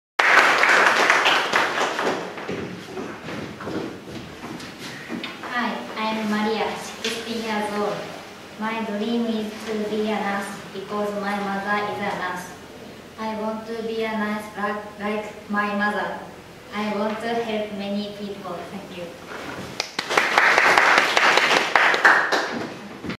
2340 Maria Watanabe
"I'm Maria, sixteen years old. My dream is to be a nurse, because my mother is a nurse. I want to be nurse like my mother. I want to help many people. Thank you."

絆 Kizuna 5: Voices of the Youth is a commemorative photo and video project presented by the The Osaka Committee of Chicago Sister Cities International for the 5th year anniversary of the Great East Japan Earthquake and Tsunami of 2011.